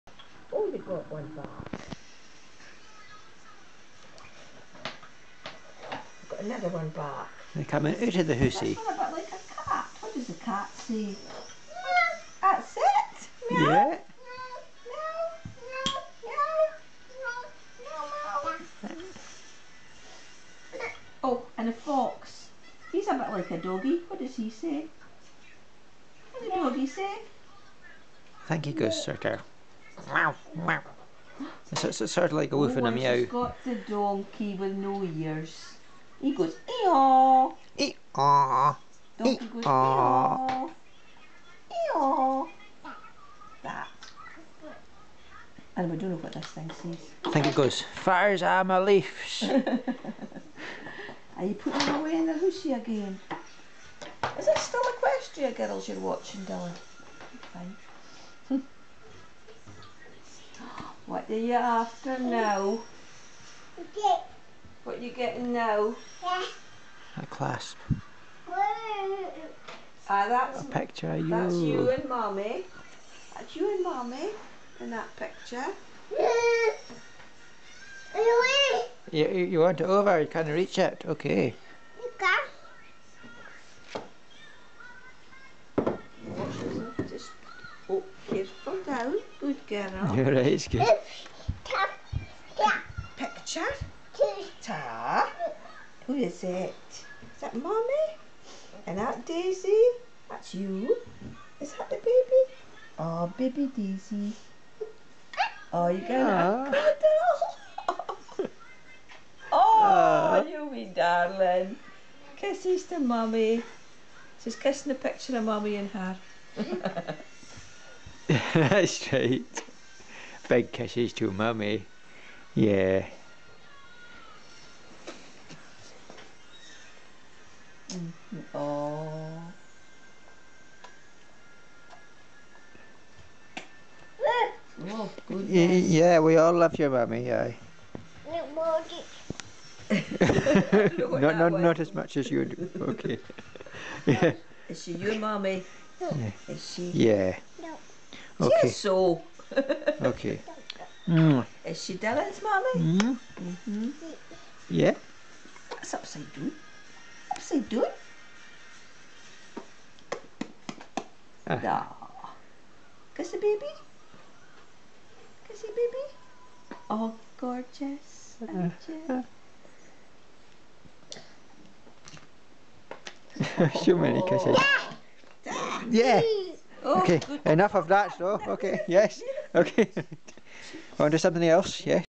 ‎May 9th ‎2017, ‏‎18:39. Daisy with her granny, looking at that picture of herself and her mum
P1050663